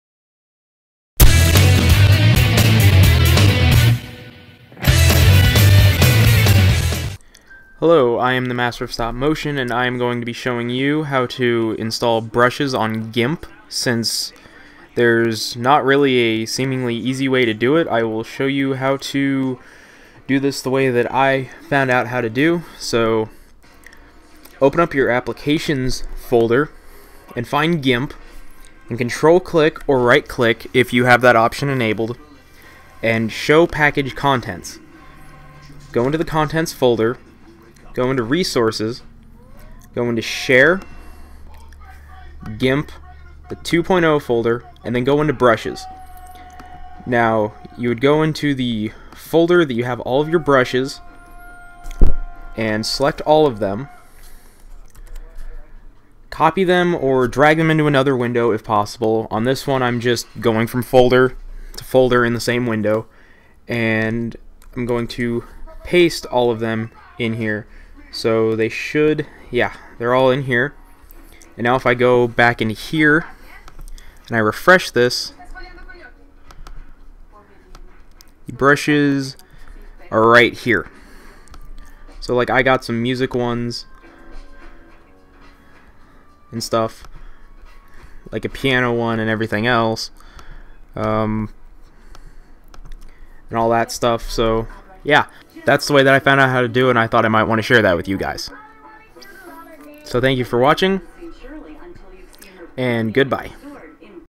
How to Install GIMP Brushes on MAC
This How-To will show you how to install burhes on GIMP on a MAC. This is just the one way that I found out how to do this. I'm sure there are other ways, but I found this way, and it's the easiest for me. I thought that other people might want to know how to do this.